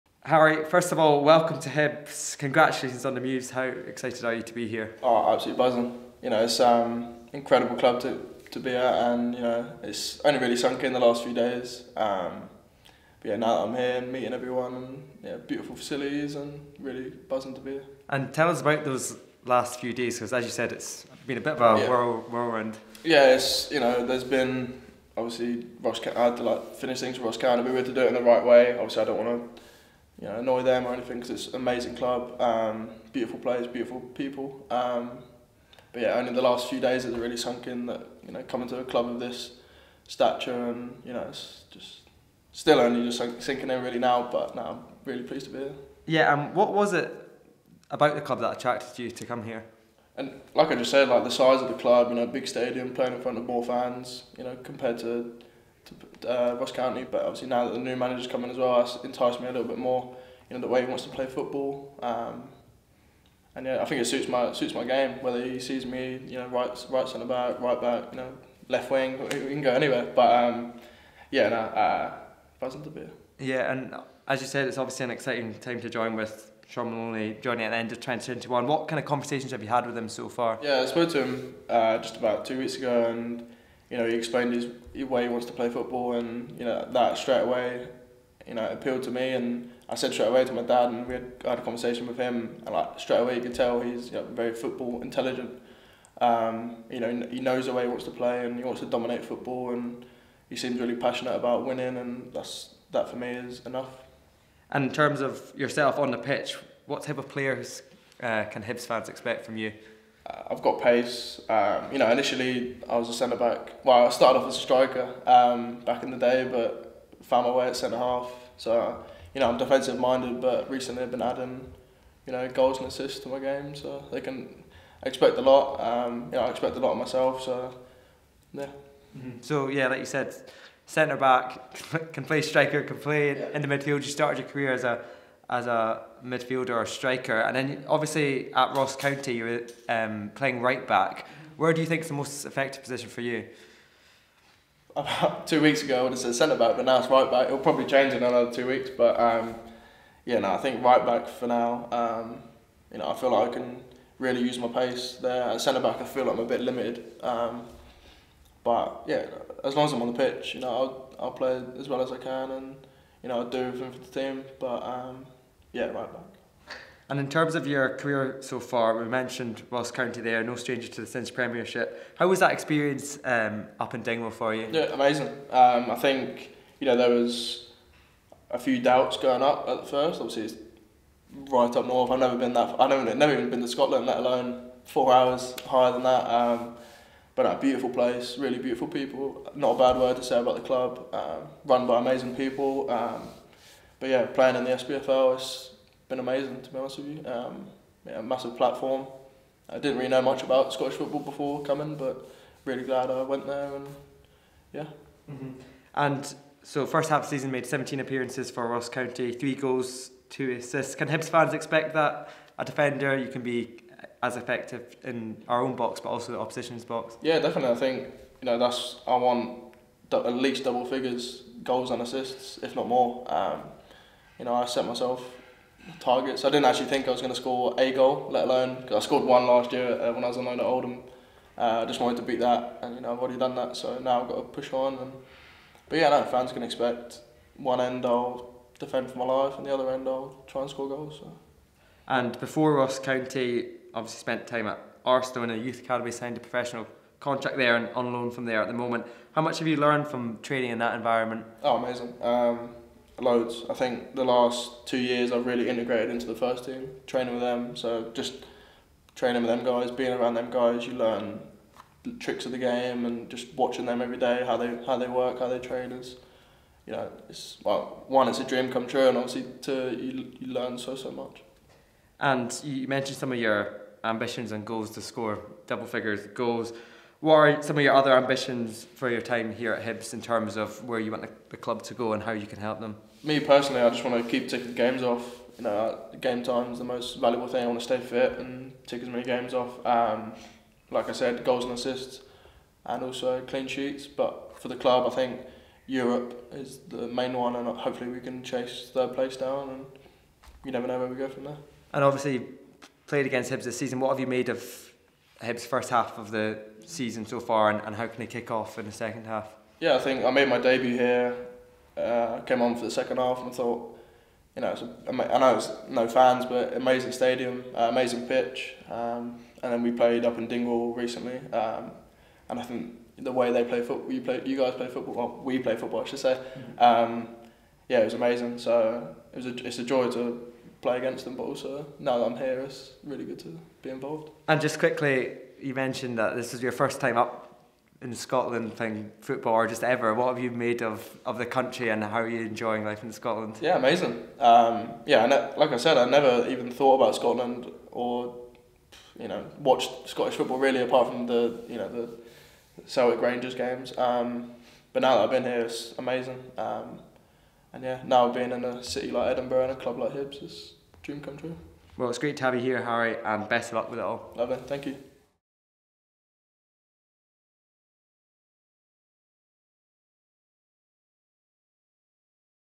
'It's Still Sinking In!' - Harry Clarke | First Hibs Interview | Hibernian FC
Hibernian new boy Harry Clarke speak in-depth to Hibs TV for the first time to discuss his move to Edinburgh and his career so far!